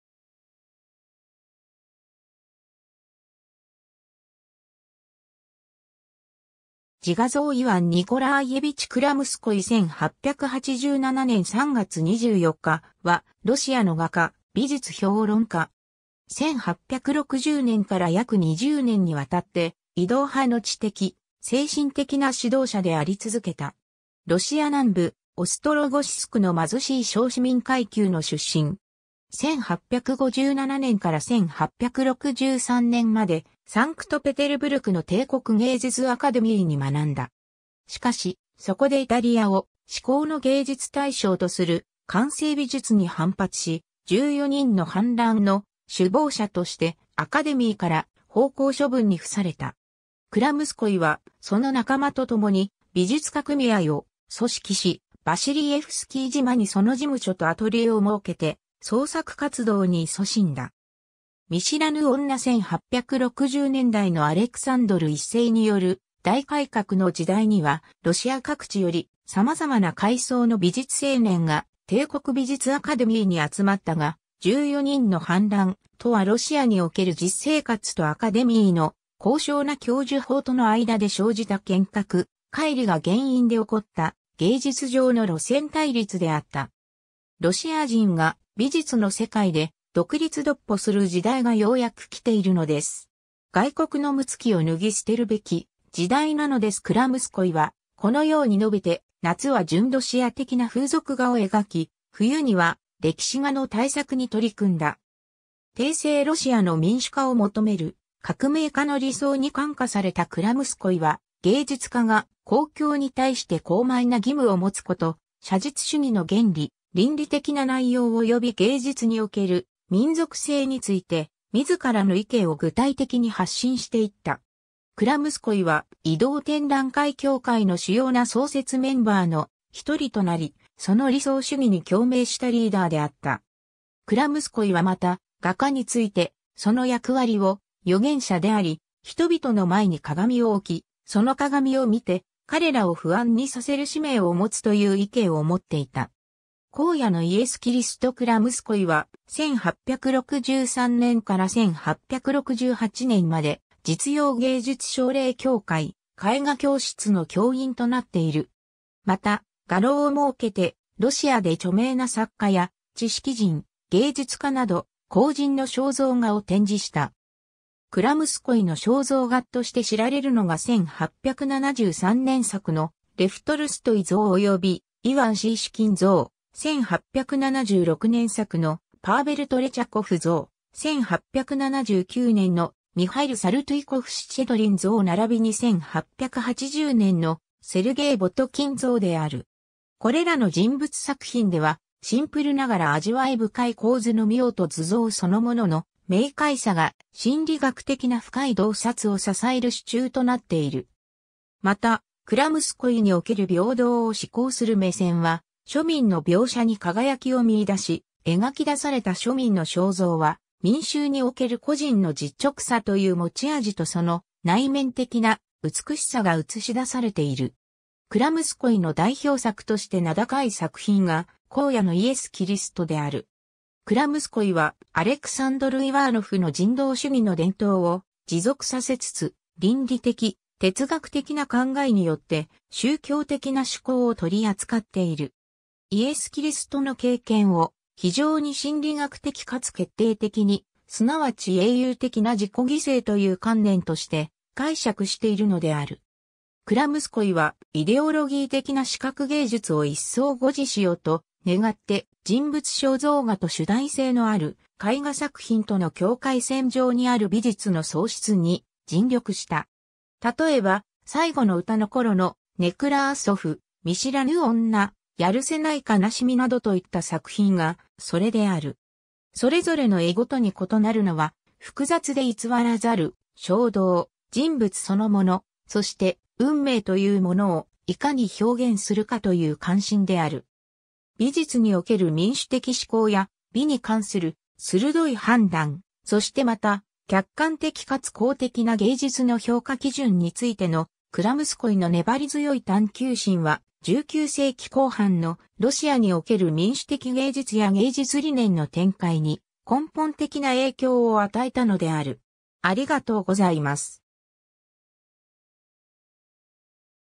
イワン・クラムスコイ
アレクサンドル・ネフスキー大修道院に埋葬された人物
自画像 イワン・ニコラーイェヴィチ・クラムスコイ（Ива́н Никола́евич Крамско́й, 1837年5月27日（グレゴリオ暦6月8日） - 1887年3月24日（グレゴリオ暦4月5日））は、ロシアの画家・美術評論家。
1860年から約20年にわたって「移動派」の知的・精神的な指導者であり続けた。
 ロシア南部オストロゴシスクの貧しい小市民階級の出身。
1857年から1863年までサンクトペテルブルクの帝国芸術アカデミー（現サンクトペテルブルク美術大学）に学んだ。
しかし、そこでイタリアを至高の芸術対象とする官製美術に反撥し、「14人の反乱」の首謀者としてアカデミーから放校処分に付された。
クラムスコイは、その仲間とともに「美術家組合 "Артель художников"」を組織し、ヴァシリエフスキー島にその事務所とアトリエを設けて創作活動にいそしんだ。
 『見知らぬ女』 1860年代のアレクサンドル1世による「大改革」の時代には、ロシア各地よりさまざまな階層の美術青年が帝国美術アカデミーに集まったが、「14人の反乱」とはロシアにおける実生活とアカデミーの「高尚な」教授法とのあいだで生じた懸隔・乖離が原因で起こった芸術上の路線対立であった。
「ロシア人が美術の世界で独立独歩する時代がようやく来ているのです。外国のむつきを脱ぎすてるべき時代なのです」－クラムスコイはこのように述べて、夏は純ロシア的な風俗画を描き、冬には歴史画の大作に取り組んだ。
 帝政ロシアの民主化を求める革命家の理想に感化されたクラムスコイは、芸術家が公共に対して高邁な義務をもつこと、写実主義の原理、倫理的な内容および芸術における民族性についてみずからの意見を具体的に発信していった。
クラムスコイは「移動展覧会協会」（もしくは「移動派」）の主要な創設メンバーのひとりとなり、その理想主義に共鳴したリーダーであった。
クラムスコイはまた、画家について、その役割を、「預言者」であり「人々の前に鏡を置き、その鏡を見て彼らを不安にさせる使命」をもつという意見をもっていた。
 『荒野のイエス・キリスト』 クラムスコイは、1863年から1868年まで実用芸術奨励協会・絵画教室の教員となっている。
また、画廊を設けて、ロシアで著名な作家や知識人、芸術家など、公人の肖像画を展示した。
クラムスコイの肖像画っとして知られるのが1873年作の「レフ・トルストイ」像および「イワン・シーシキン」像、1876年作の「パーヴェル・トレチャコフ」像、1879年の「ミハイル・サルトゥイコフ＝シチェドリン」像ならびに1880年の「セルゲイ・ボトキン」像である。
これらの人物作品では、シンプルながら味わい深い構図の妙と図像そのものの明快さが、心理学的な深い洞察をささえる主柱となっている。
また、クラムスコイにおける平等を指向する目線は、庶民の描写に輝きを見出し、描き出された庶民の肖像は、民衆における個人の実直さという持ち味とその内面的な美しさが映し出されている。
 クラムスコイの代表作として名高い作品が『荒野のイエス・キリスト』（1872年、トレチャコフ美術館蔵）である。
クラムスコイは、アレクサンドル・イワーノフの人道主義の伝統を持続させつつ、倫理的・哲学的な考えによって宗教的な趣向を取り扱っている。
イエス・キリストの経験を、非常に心理学的かつ決定的に――すなわち英雄的な自己犠牲という観念として――解釈しているのである。
 クラムスコイは、イデオロギー的な視覚芸術をいっそう護持しようと願って、人物肖像画と主題性のある絵画作品との境界線上にある美術の創出に尽力した。
たとえば、「『最後の詩』の頃のネクラーソフ (И.Н. Некрасов в период "Последних песен")」（［1877年-78年］）、「見知らぬ女」（［1883年］）、「遣る瀬ない悲しみ」（［1884年］）などといった作品が、それである。
それぞれの絵ごとに異なるのは、複雑で偽らざる衝動、人物そのもの、そして、運命というものをいかに表現するかという関心である。
 美術における民主的志向や、美に関する鋭い判断、そしてまた、客観的かつ公的な芸術の評価基準についてのクラムスコイの粘り強い探究心は、19世紀後半のロシアにおける民主的芸術や芸術理念の展開に、根本的な影響を...